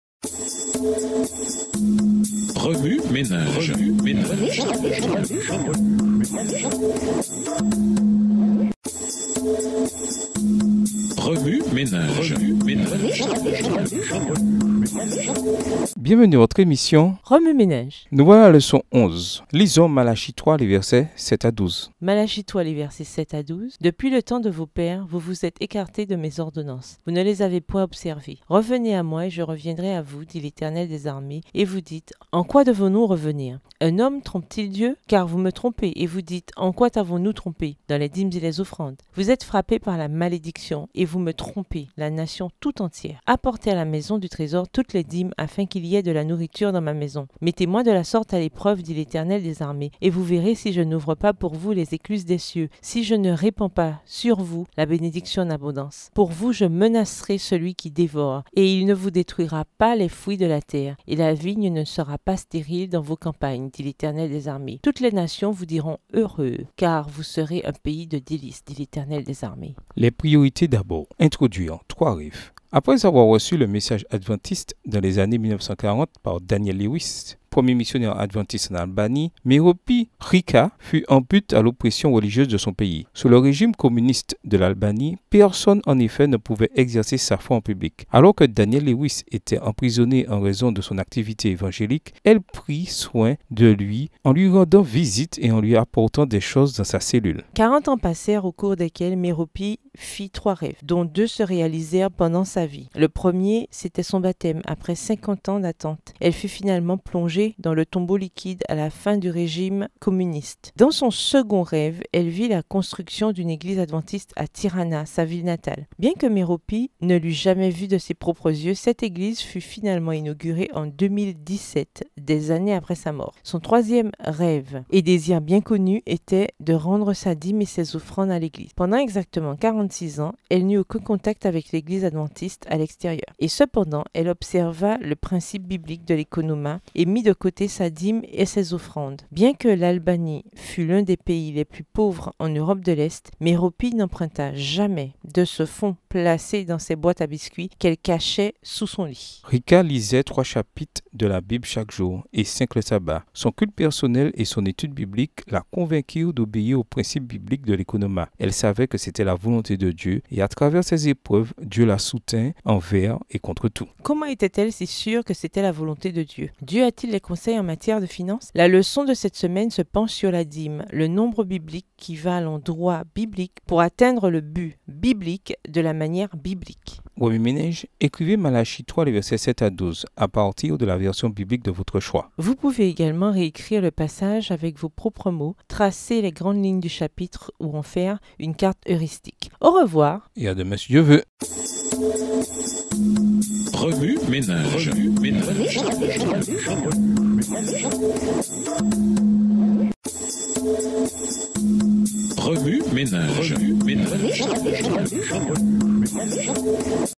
Leçon 11 : Questionnaire JA, Sabbat après-midi 6 Juin, Trois rêves
Remue-méninges
Écrivez Malachie 3. 7-12 à partir de la version biblique de votre choix
Vous pouvez également réécrire le passage avec vos propres mots, tracer les grandes lignes du chapitre ou en faire une mind map